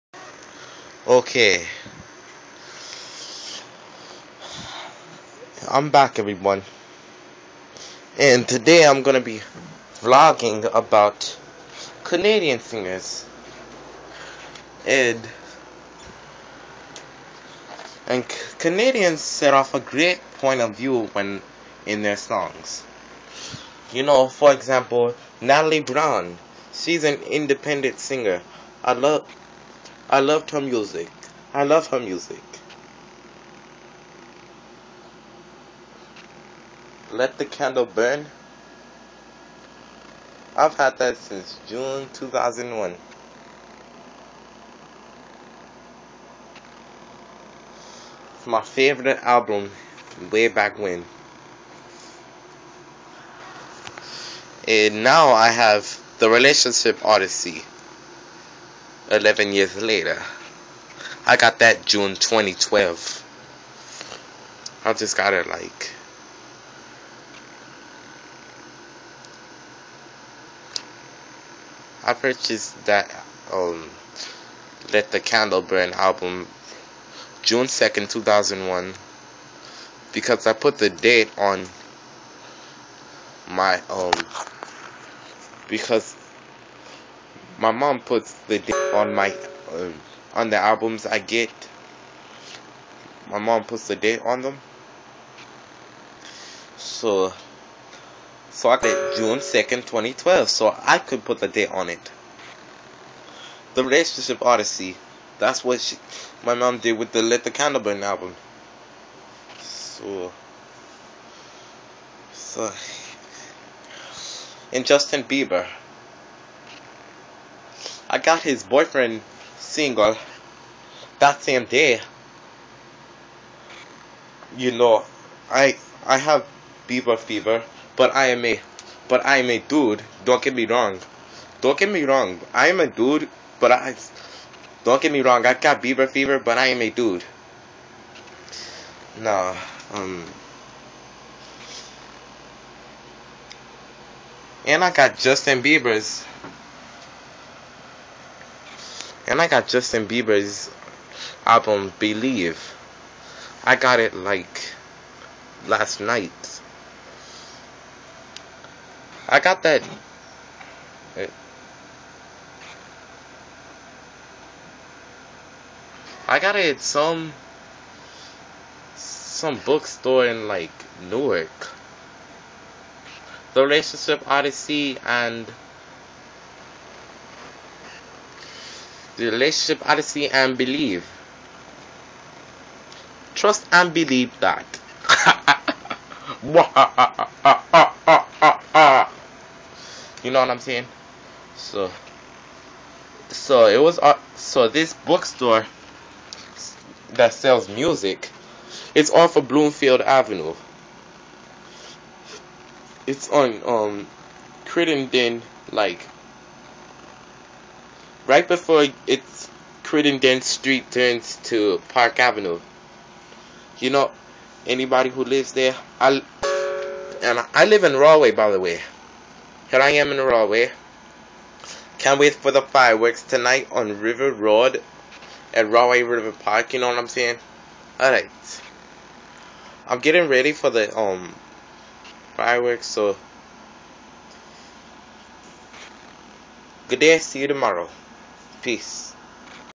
Vlog 10: Canadian Singers Are Great
This is for Natalie Brown, Justin Bieber, Carly Rae Jepsen, Keshia Chante & other Canadian singers out there.